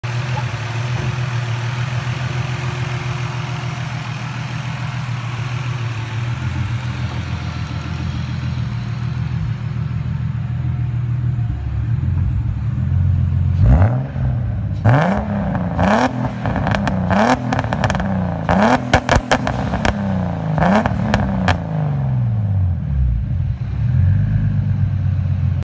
BMW M3 Turbo 3.5" straight through exhaust system with Magnaflow muffler.
This is the current status of my turbo M3 project. I have installed a new 4.5" intercooler and new 3" Aluminium charge piping. The waste gate was also touted back into the exhaust which makes it sound much better.